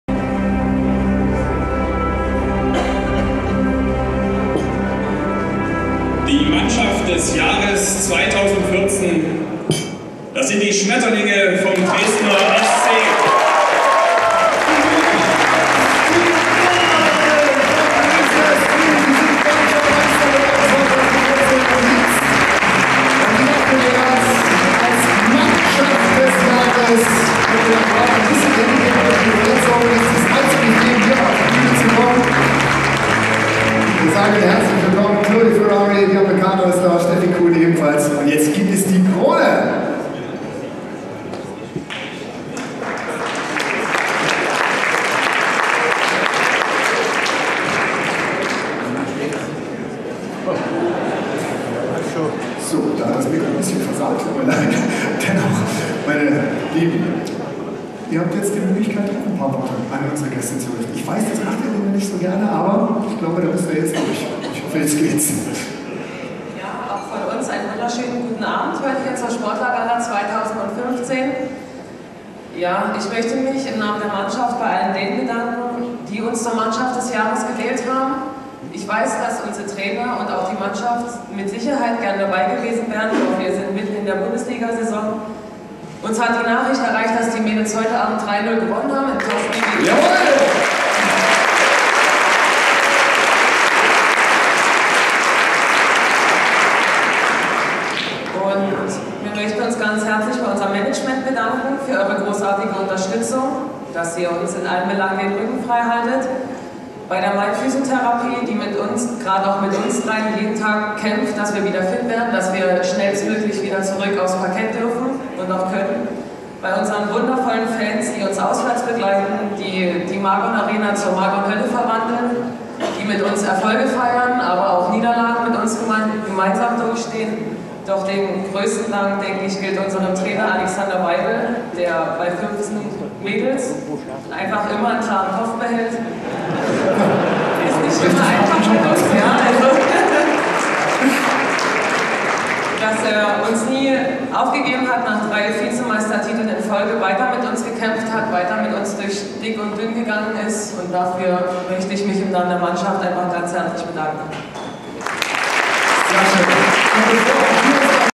Herzlichen Glückwunsch an die DSC Volleyball Damen
Die Volleyballerinnen wurden am Samstagabend in Dresden als Mannschaft des Jahres gekürt. Hier ein Ausschnitt von der Sächsische Sportgala 2015.